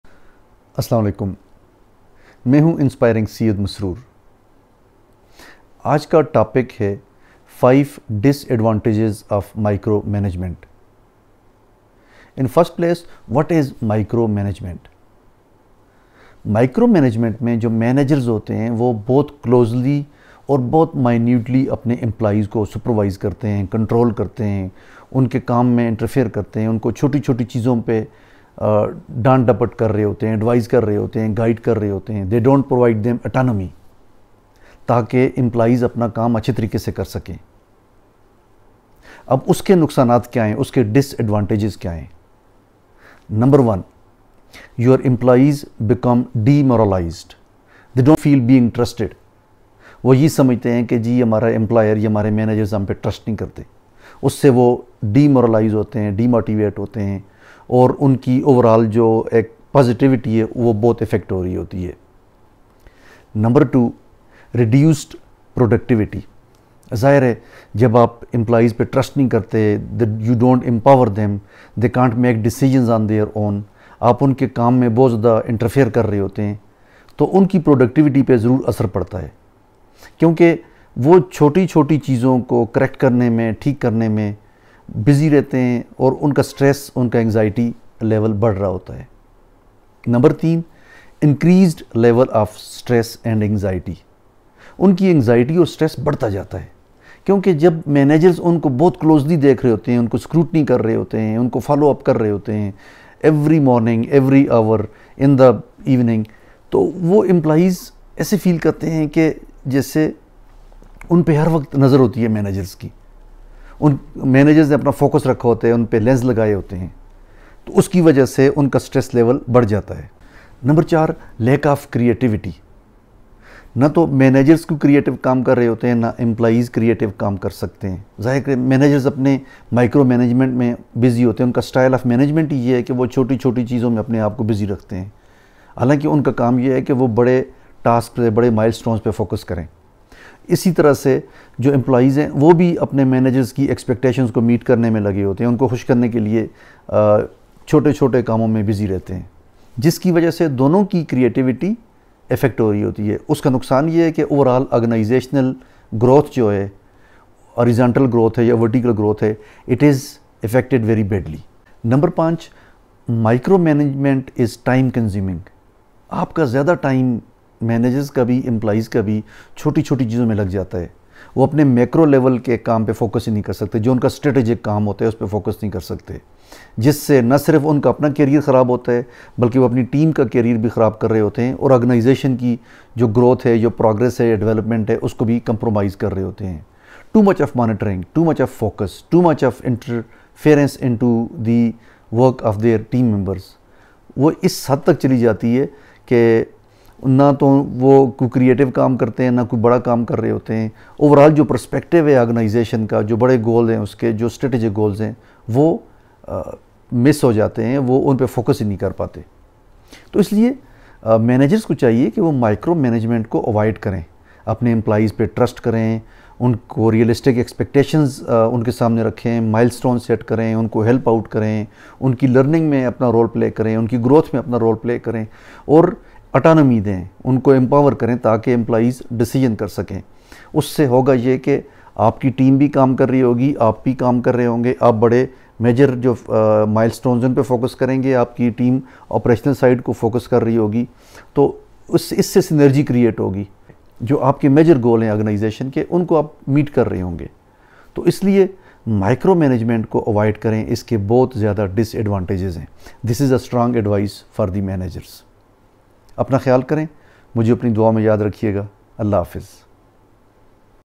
5 Disadvantages of Micromanagement | Inspiring Syed Masroor
5 Disadvantages of Micromanagement!
Micromanagement leads to closely and minutely control and manage the employees. It is a non-progressive management style. It has many disadvantages but inspiring Syed Masroor will talk about five significant disadvantages of micromanement. Please watch this video to learn more about the topic.

Was this response better or worse?
Better
Worse
Same